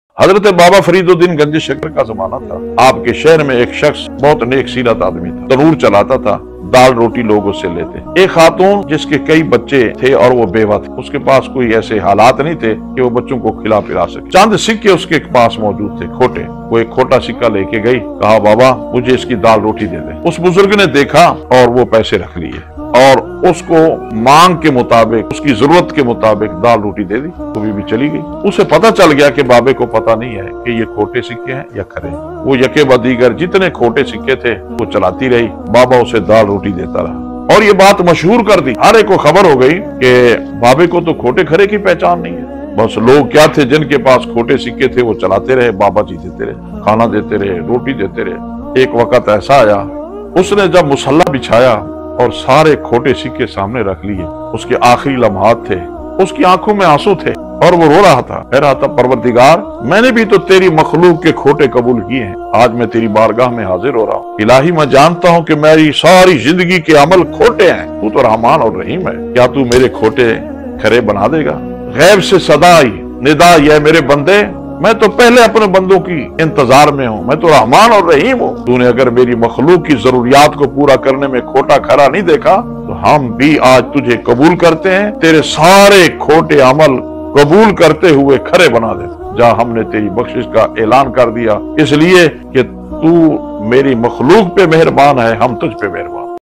کھوٹے عمل کیسے قبول ہوں گے | M RAFIQ SHAH JAMALI | ISLAMIC VIDEO
ALLAH HAMARY AMAL KESY QABOOL KARY GA